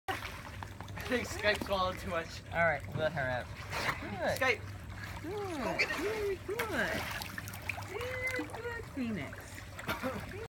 Afghan Puppy Phoenix swims in pool
6 month old puppy finds a pool a great place to cool off